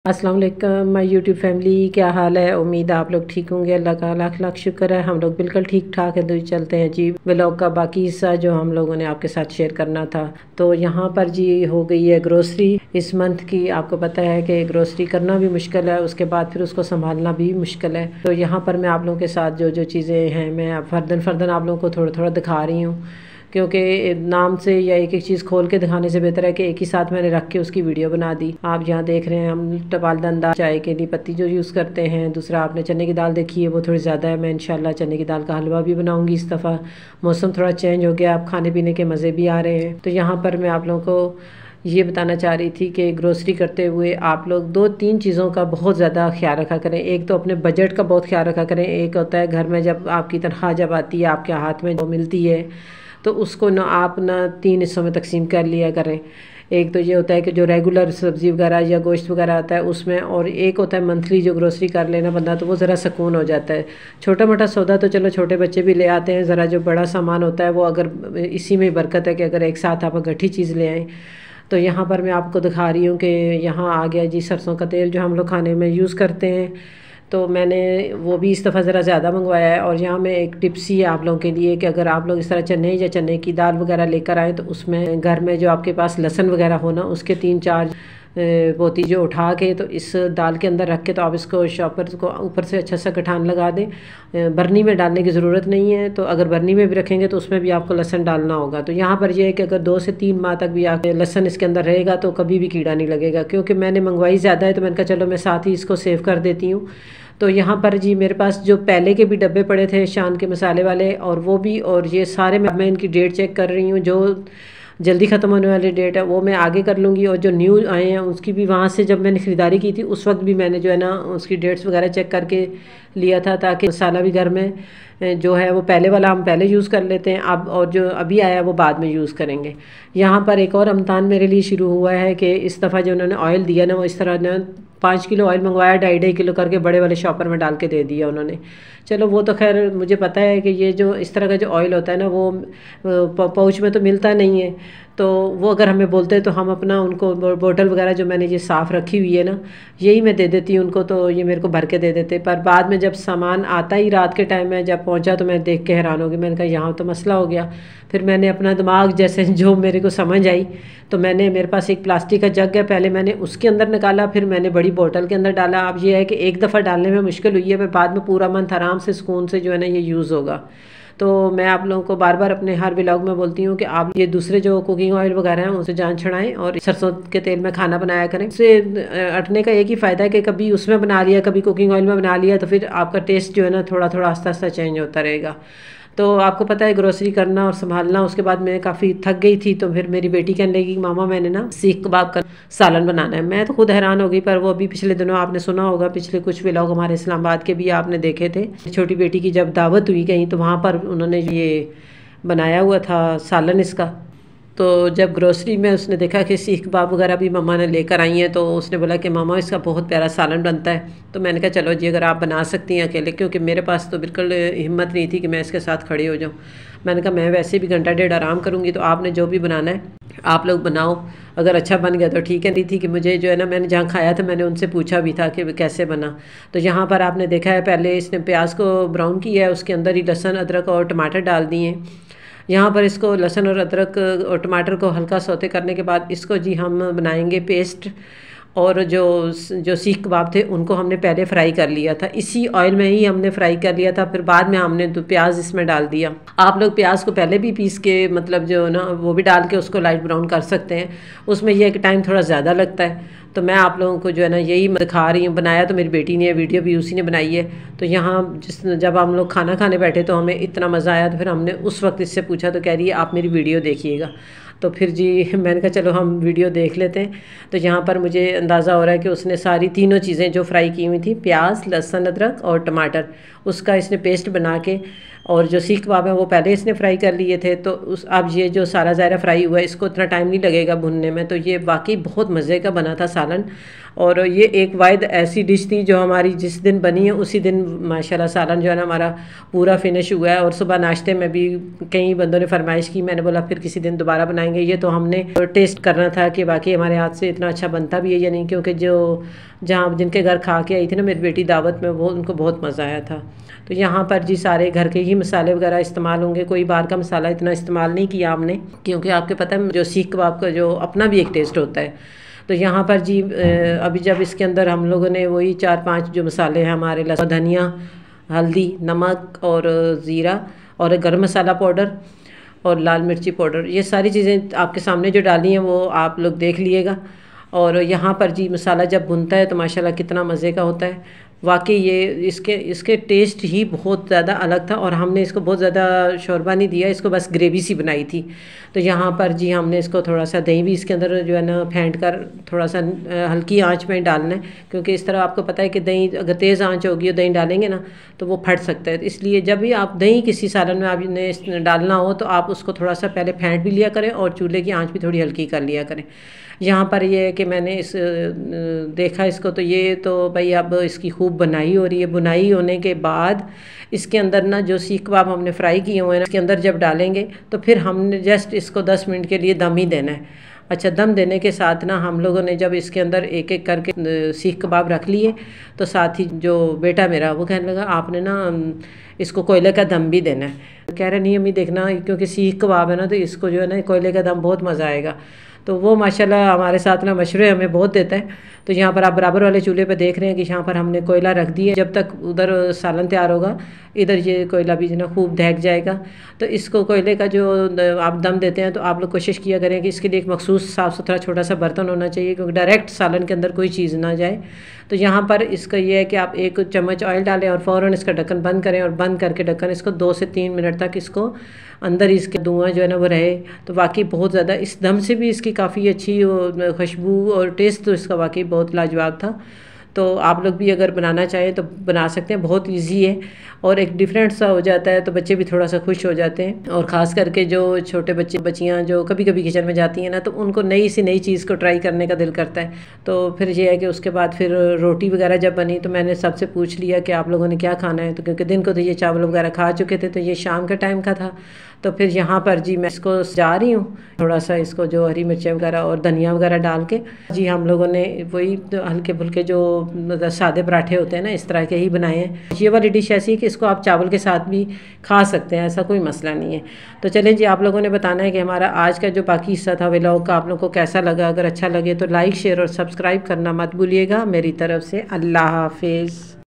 Monthly Grocery Vlog | Delicious Seekh Kabab With Gravy Recipe | FM home
In this video, I am sharing with you all an amazing video of monthly grocery and then I am sharing the best recipe of Delicious seekh kabab with gravy. This is a flavourful twist to the taste of seekh kabab and you will defined love the recipe of seekh kabab gravy. I hope you will like my video.